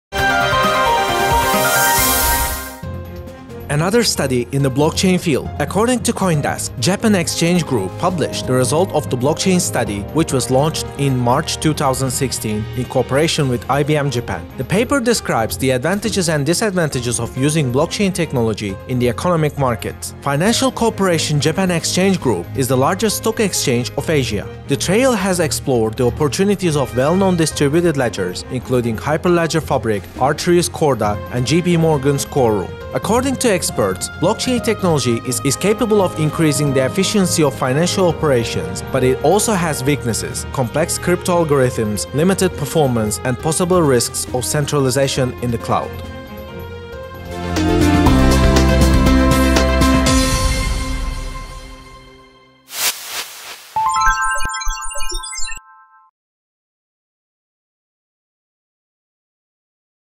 A new research of distributed ledger system
Another study in the blockchain field. According to Coindesk, Japan Exchange Group (JPX) published the results of the blockchain study, which was launched in March 2016 in cooperation with IBM JAPAN. The paper describes the advantages and disadvantages of using blockchain technology in the economic markets. Financial Corporation Japan Exchange Group is the largest stock exchange of Asia. The trail has explored the opportunities of well known distributed ledgers, including Hyperledger Fabric, R3’s Corda and JPMorgan’s Quorum. According to experts, blockchain technology is capable of increasing the efficiency of financial operations, but it also has weaknesses: complex cryptoalgorithms, limited performance and possible risks of centralization in the "cloud".